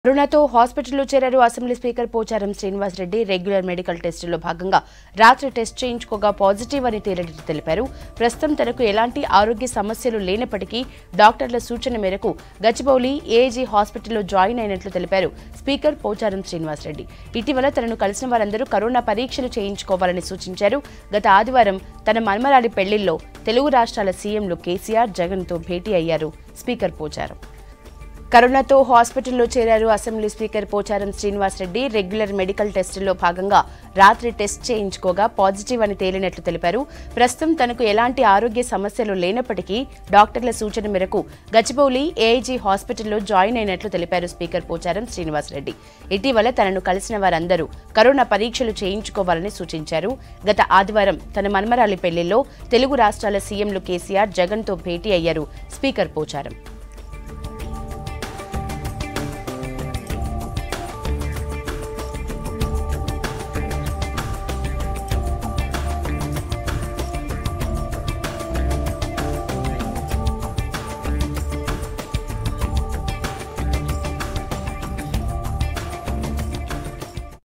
Speaker Pocharam Srinivas Reddy Tested Positive For Covid-19 | V6 News
Speaker Pocharam Srinivas Reddy Tested Positive For Covid-19